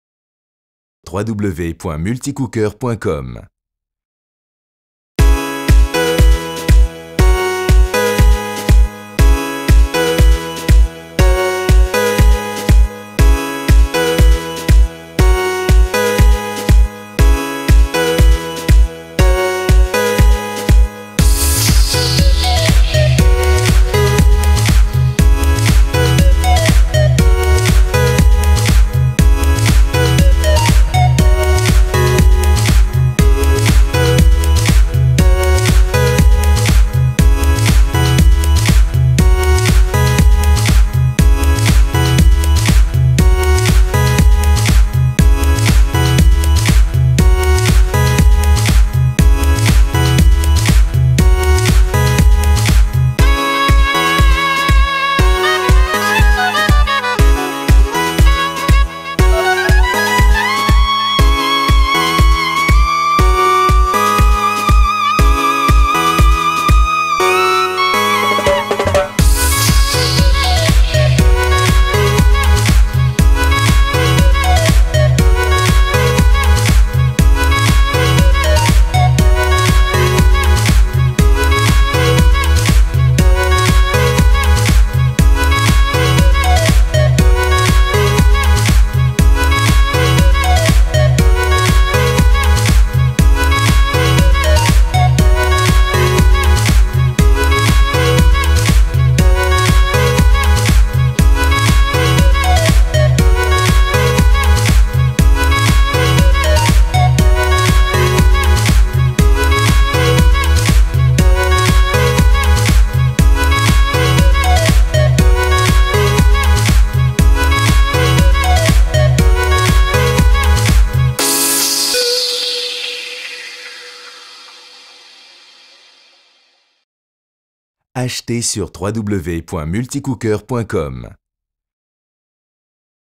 Velouté de potiron, avec le Multicuiseur REDMOND RMC-M4510E, recette
Velouté de potiron.

Ingrédients:
Potiron – 800 g
Orange – 300 g (selon votre gout)
Oignon – 100 g
Curry
Sel

Sélectionnez le programme SOUP, le temps de cuisson 40 minutes
Si vous voulez ajouter le jus d’orange,  10 minutes avant la fin du programme ouvrez le couvercle, verser le jus et mélanger. Fermez le couvercle. Laissez cuire jusqu’à la fin du programme.